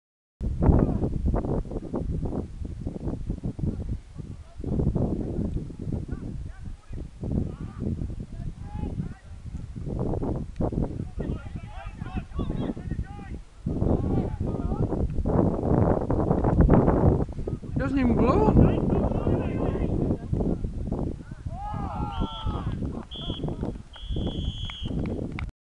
Pre-season game, Tregony 1 - 11 St Stephen Reserves
Kyle Willmott is fouled the referee ignores this then Oli Knight smashes home our 4th goal right on H/T